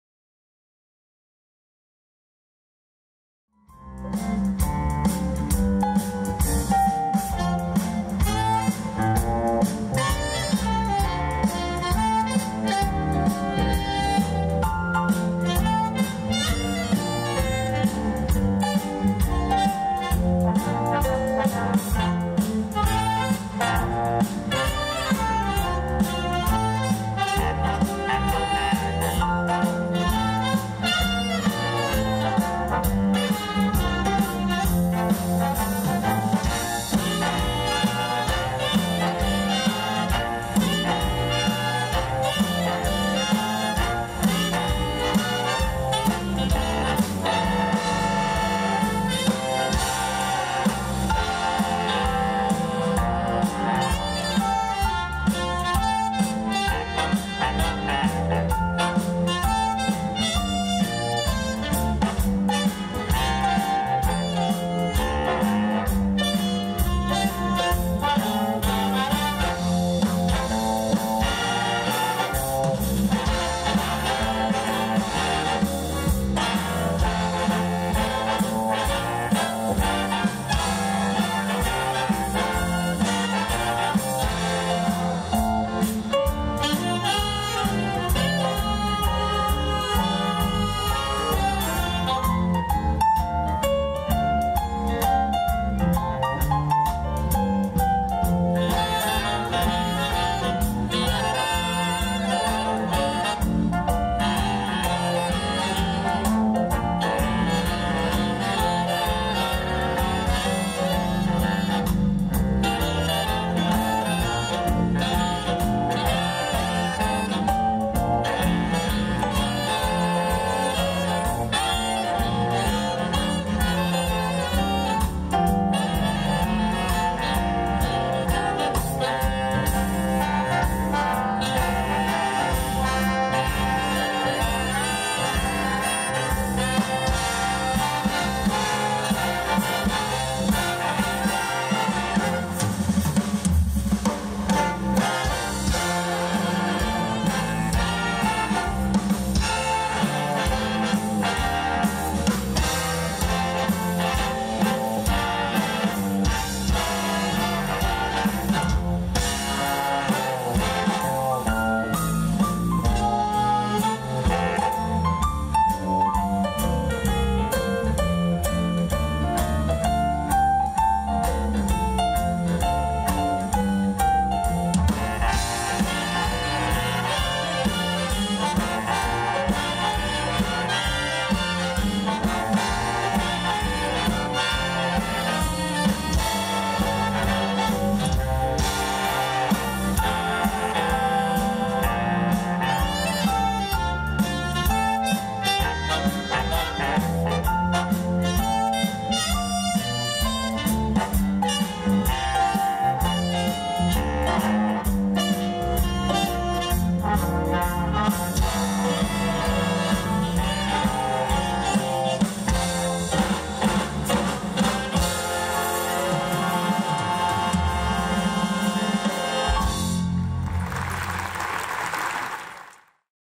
Talavera Big Band en Talavera Jazz 2023
Tuvimos el placer de contar con la Talavera Big Band en el Festival de 2023, y nos deleitaron con temas como éste, en nuestra querida Plaza del Pan.

Podéis conocer más sobre ellos en @TalaveraBigBand
Grandes profesionales de la música de orígenes talaveranos y de la comarca, con un talento magnífico sobre el escenario.